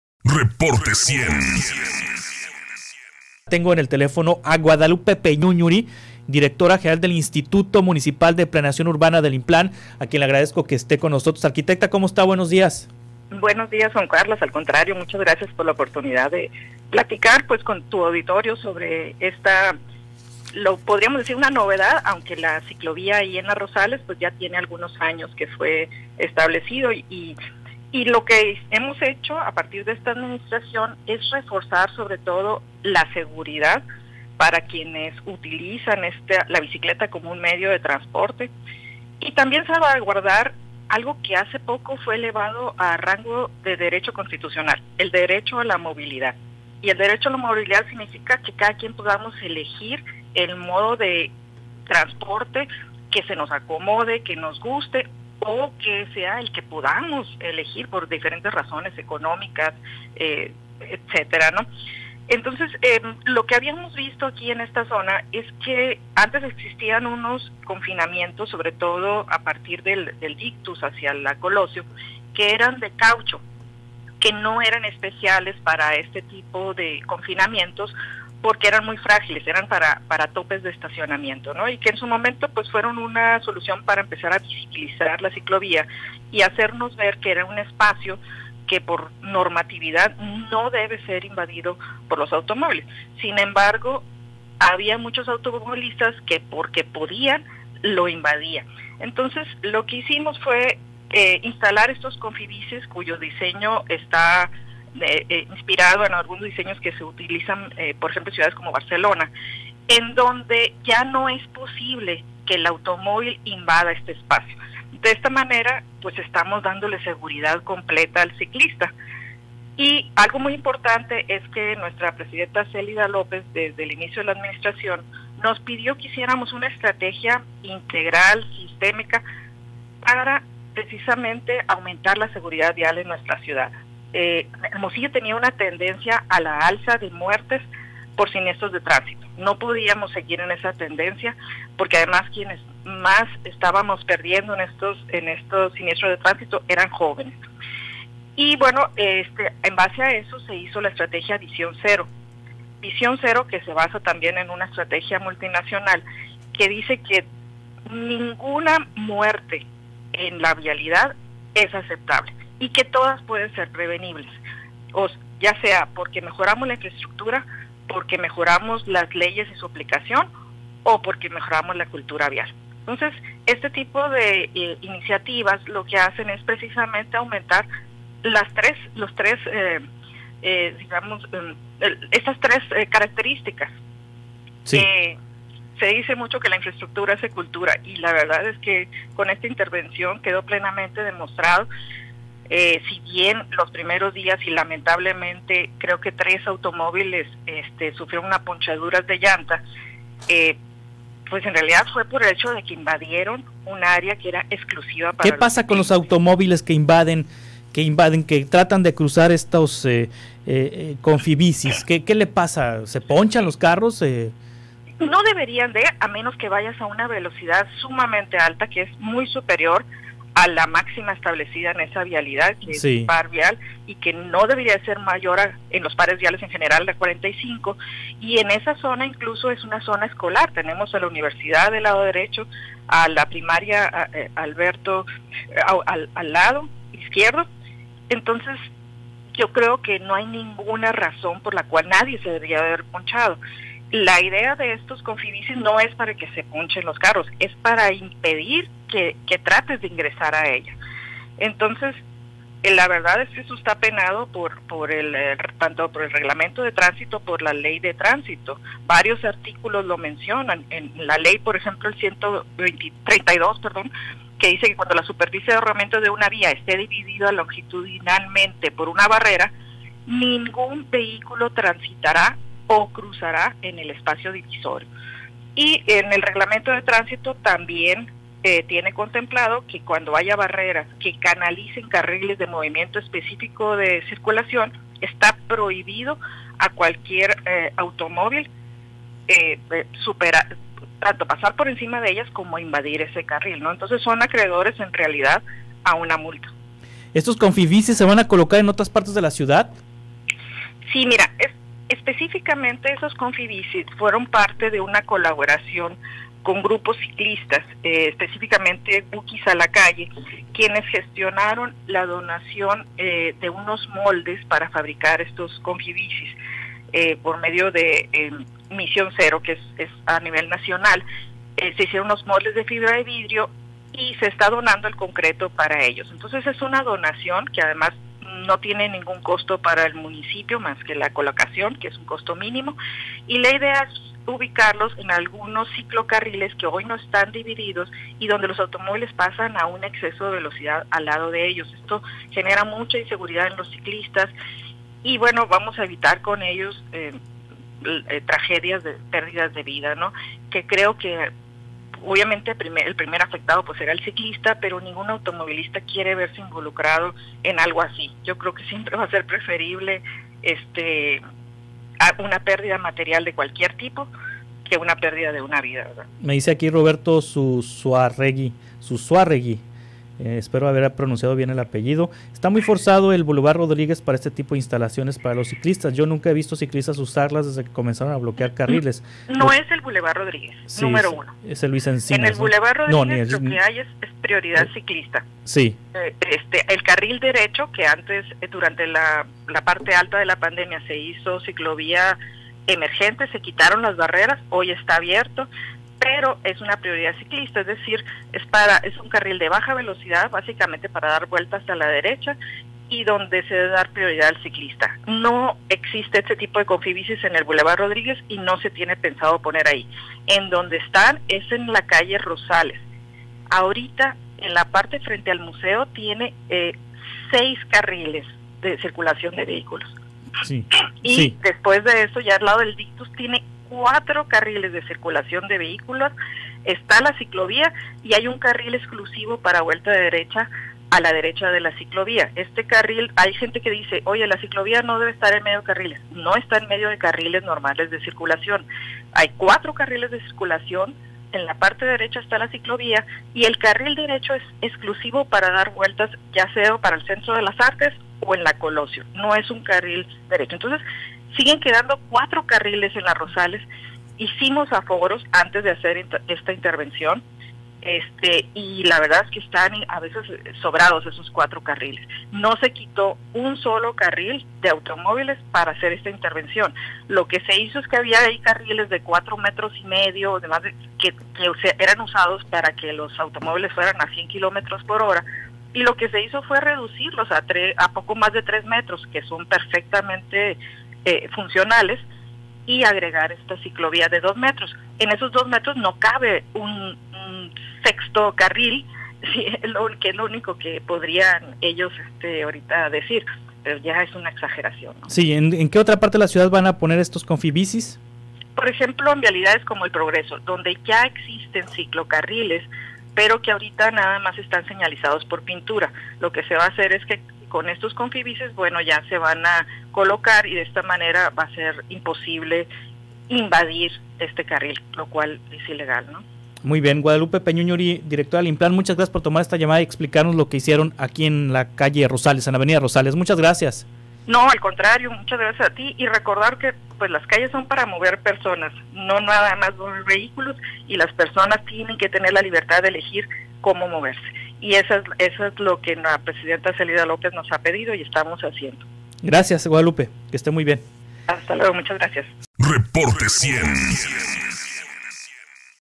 Confibicis ¿qué son y para qué sirven? | REPORTE 100
La directora general del Instituto Municipal de Planeación Urbana (Implan), Guadalupe Peñúñuri, platicó en Reporte100 con Juan Carlos Zúñiga acerca de los confibicis, ¿qué son y para qué sirven?, conflicto con automovilistas.